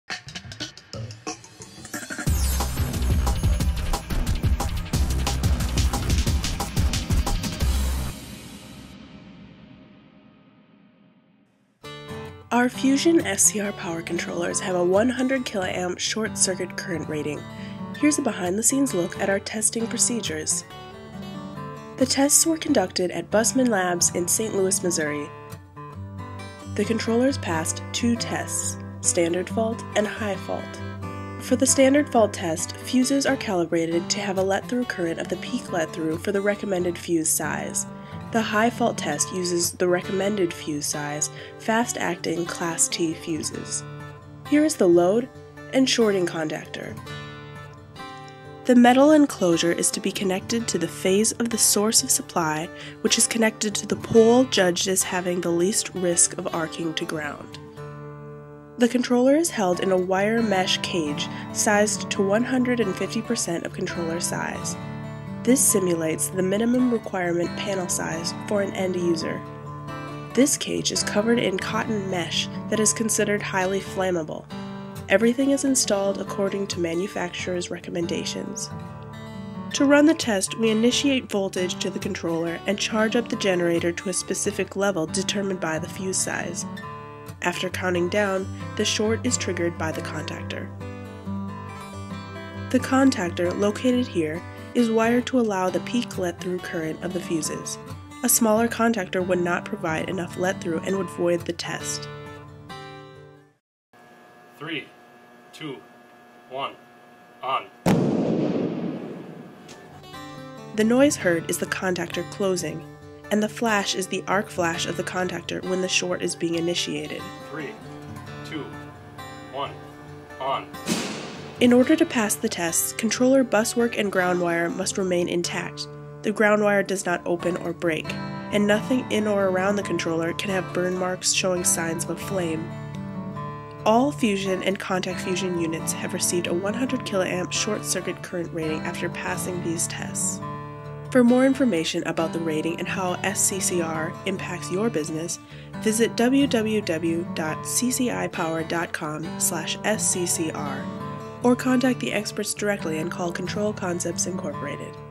SCCR Testing Procedures
All our FUSION and Compact FUSION SCR power controllers have received 100kA SCCR. Watch a brief overview of our testing procedures.